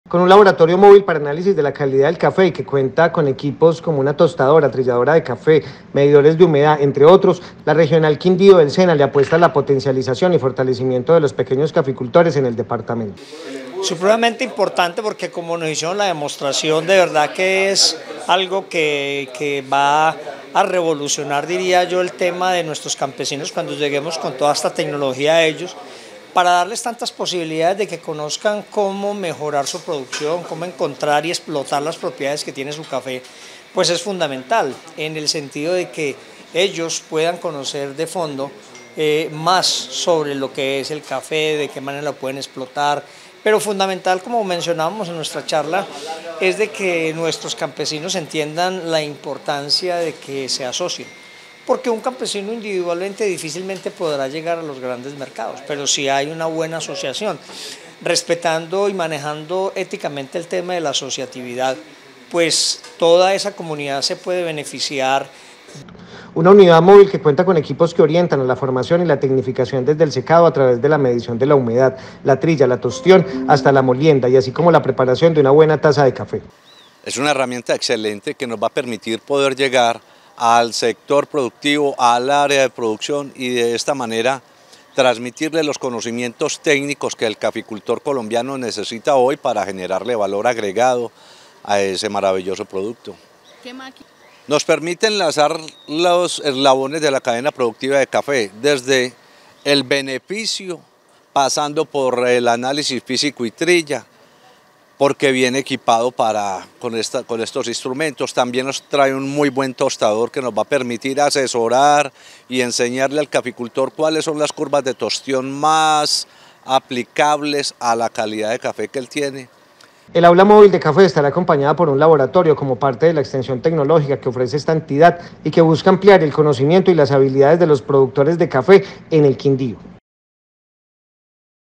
Con laboratorio móvil se impulsa la productividad de los caficultores en Quindío
Con un laboratorio móvil para análisis de la calidad del café, y que cuenta cin cuenta con equipos como tostadora de café portátil, trilladora de café pergamino, medidor de humedad, entre otros. la Regional Quindío del SENA;  le apuesta a la potencialización y fortalecimiento de los pequeños caficultores del departamento.